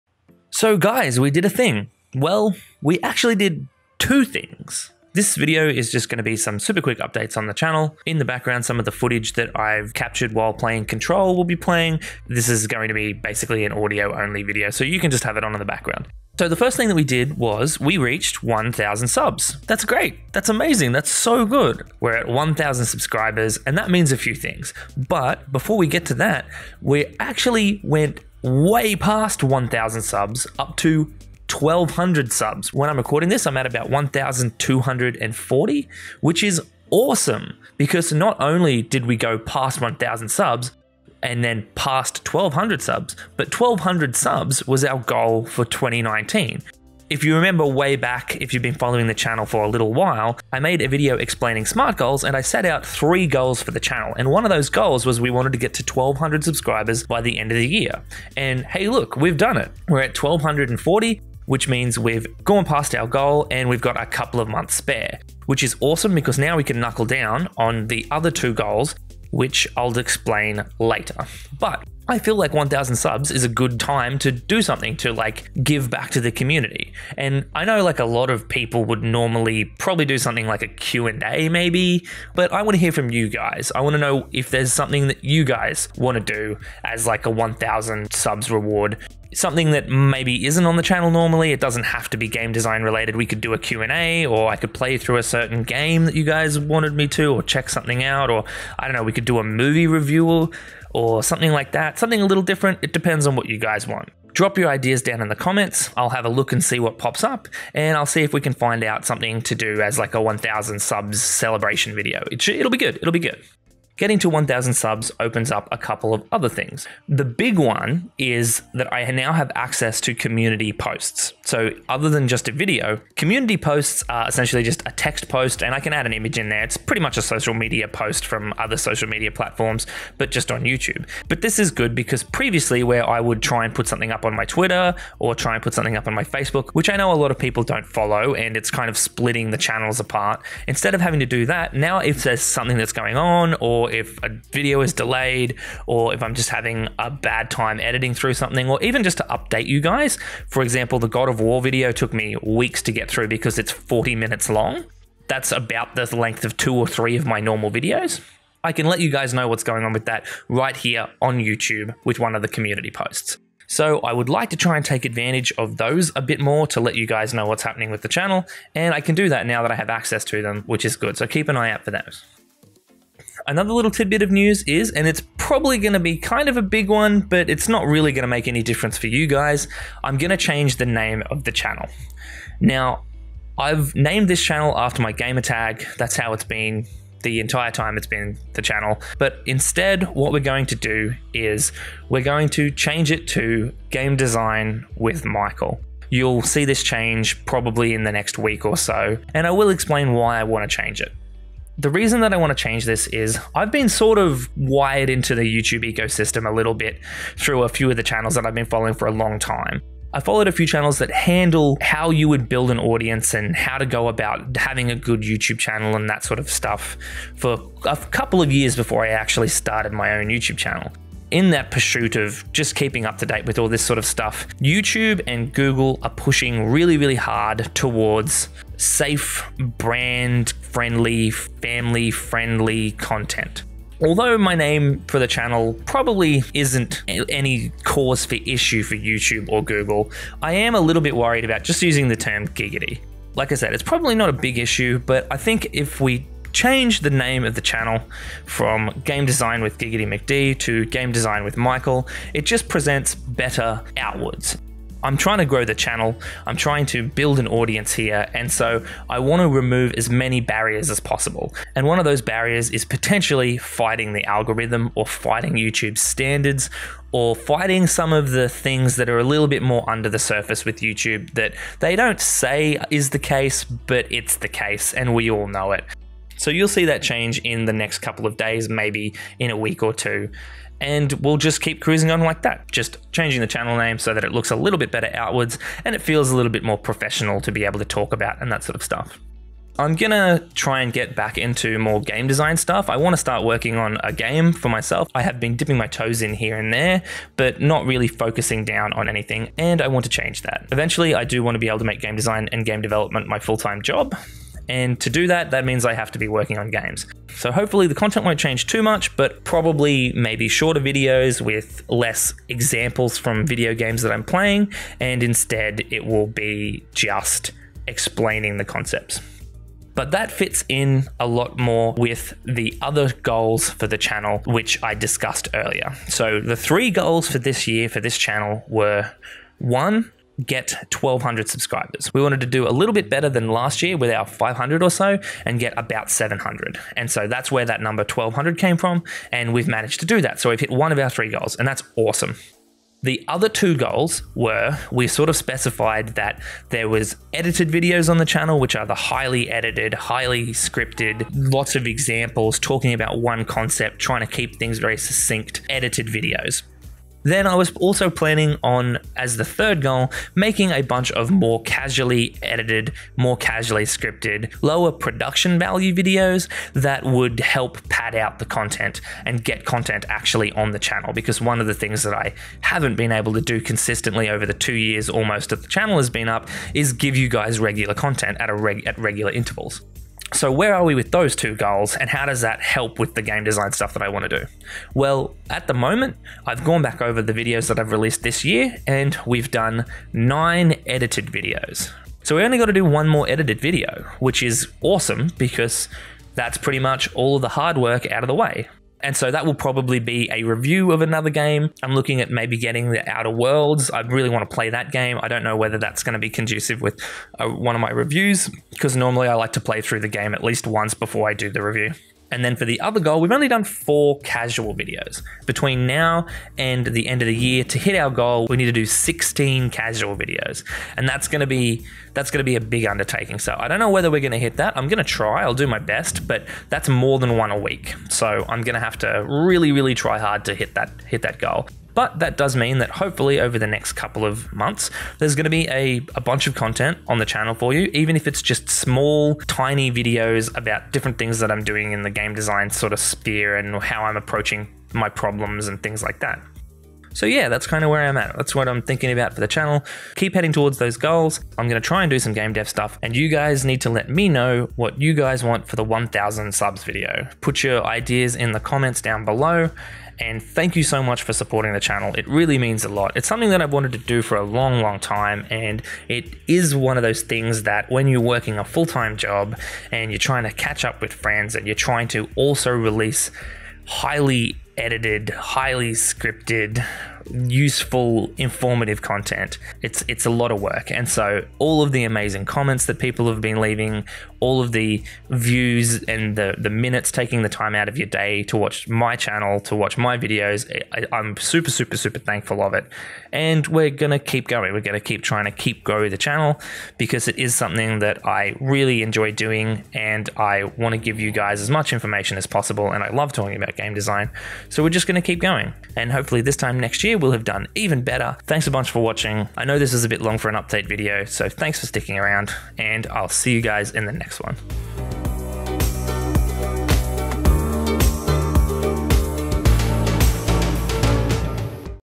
I want to hear from you....... channel name change, 1000 subs & more
Some updates for the channel around hitting 1000 subscribers, changing the name of the channel and where we are sitting with our goals for the year.